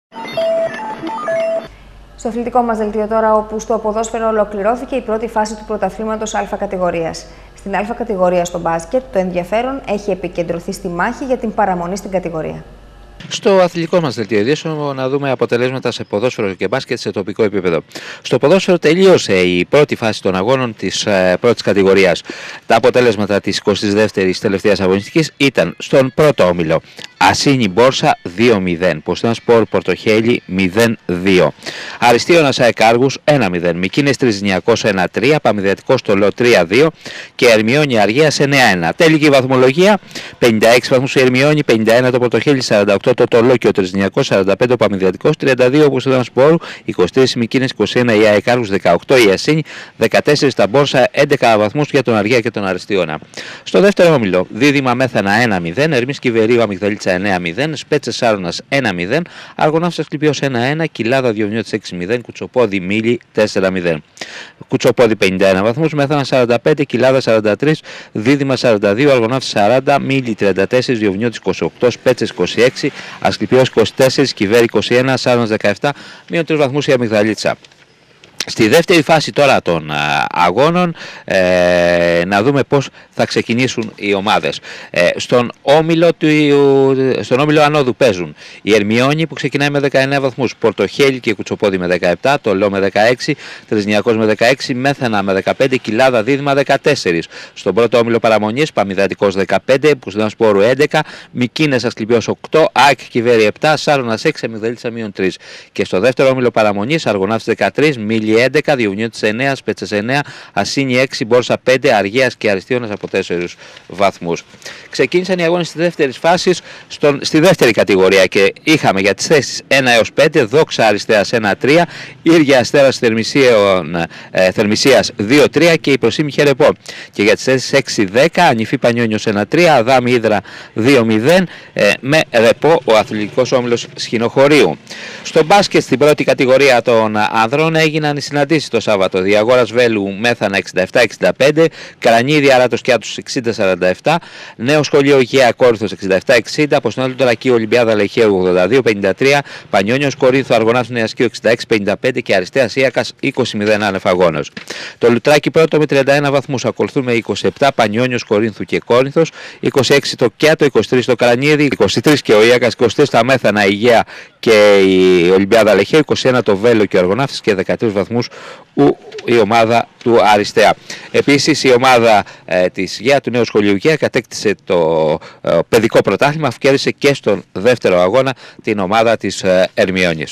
ΑΡΓΟΛΙΔΑ 2-3-2011 DRTV
Αργολίδα - Τα αποτελέσματα σε τοπικό επίπεδο.